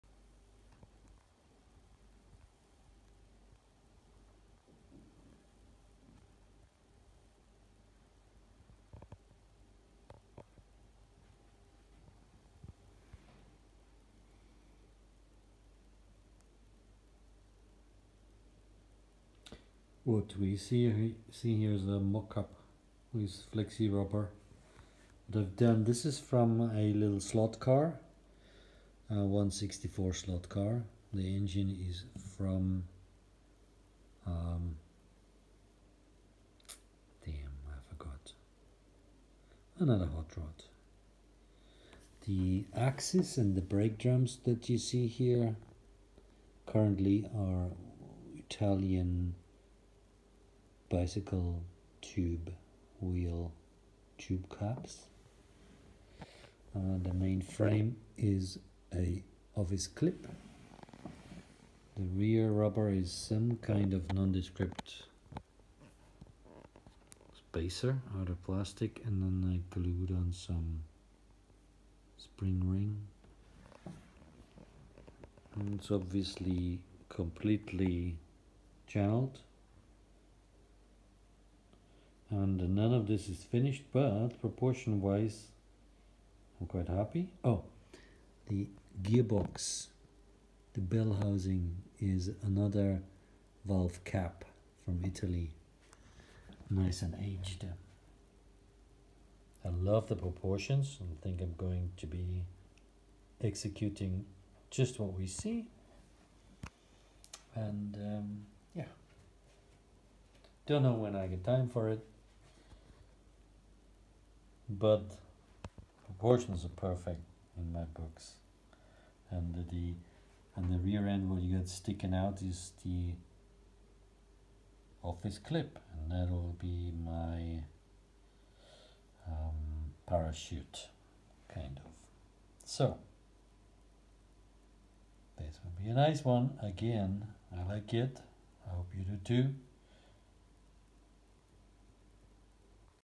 HotWHeeLs ' 32 Ford Boneshaker engine Custom HOtRod "Zoe" mockup
Testing some parts for overall proportions.
With gearbox bell housing (Vintage Bicycle Valve Cap)